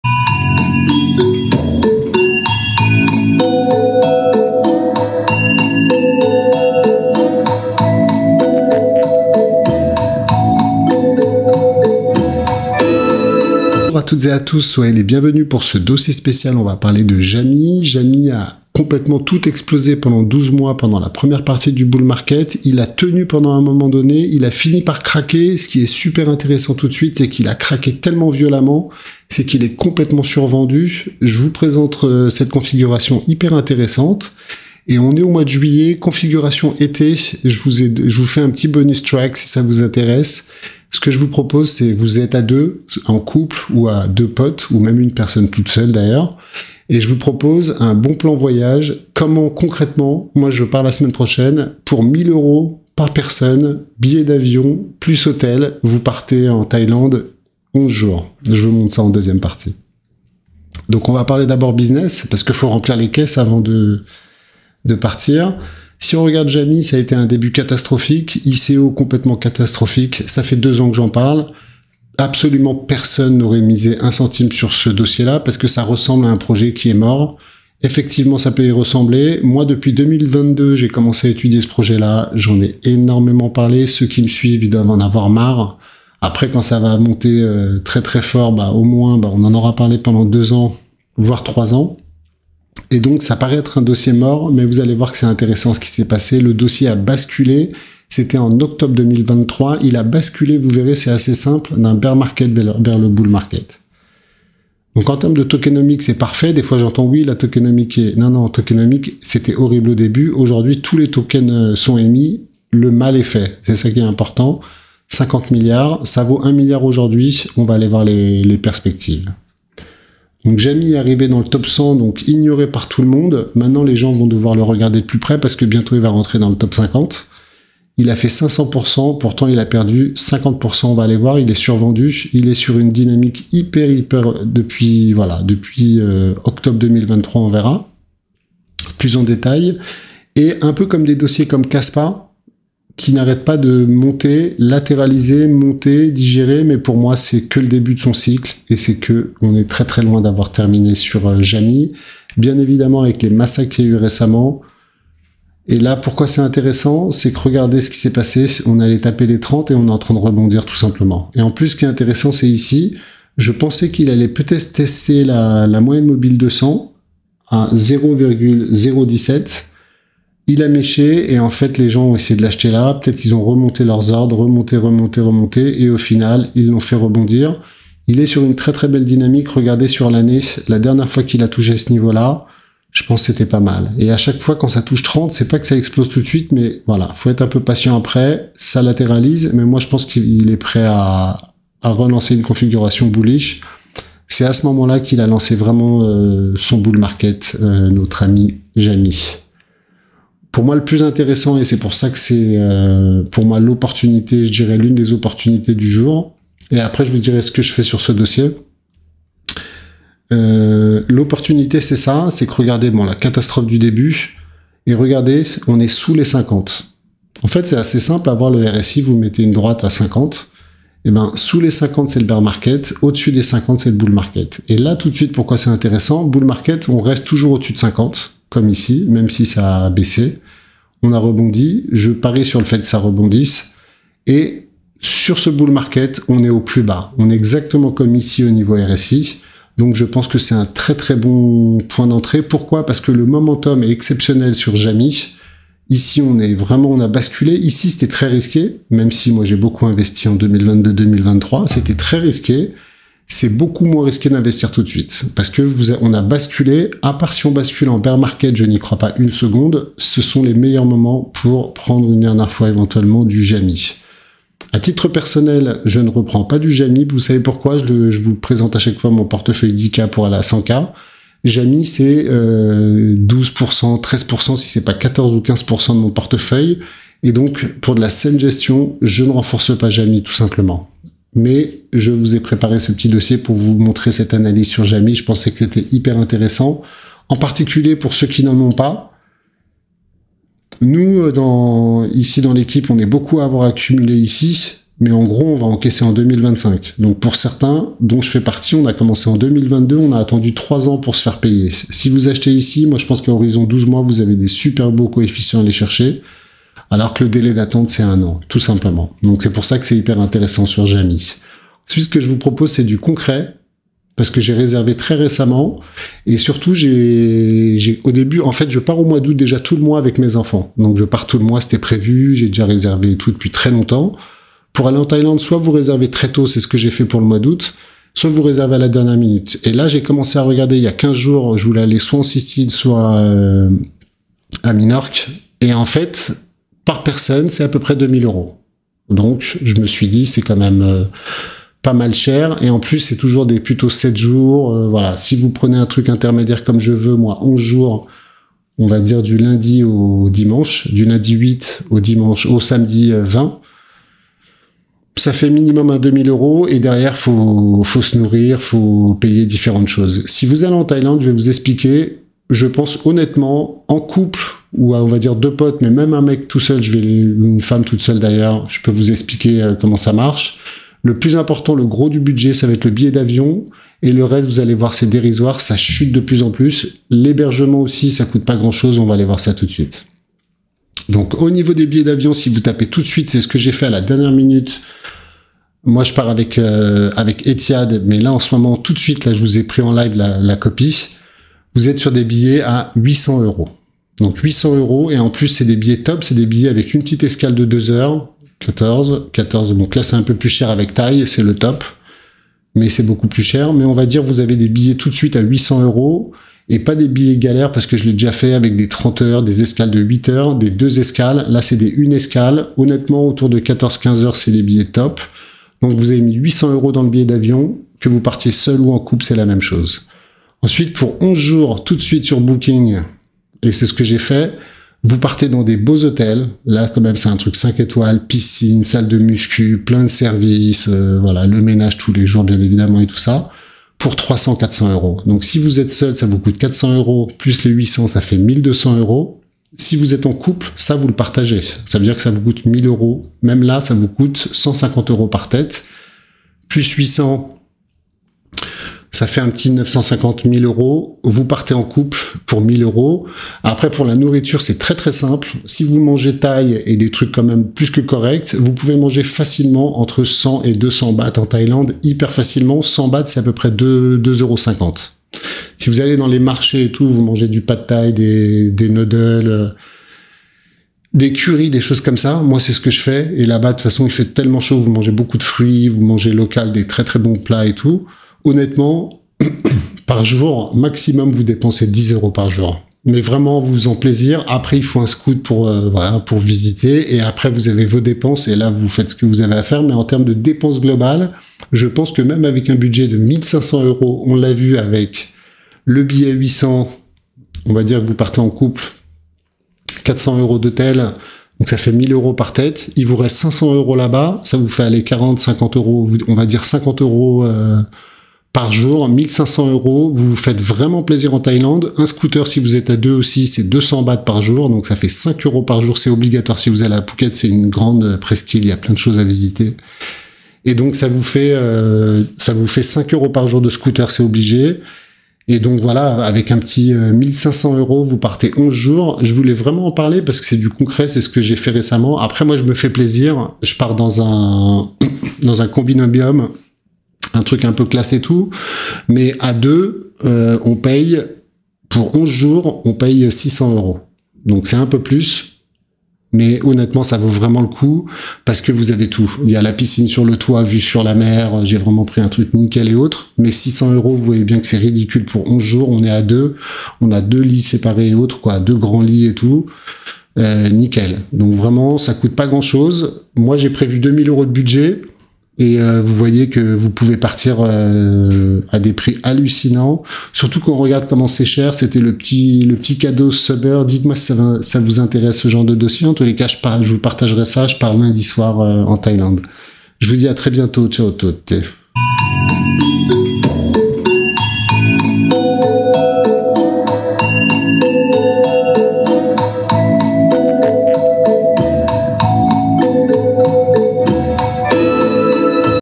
Jamais sans JASMY 🔥 EXCLU 💡 Voyages ✈️ Je pars 11 jours en Thailande 🇹🇭 pour 1 000€ Avion + Hotel 👍
0:00 Jasmy
5:00 Dossier Thailande pratique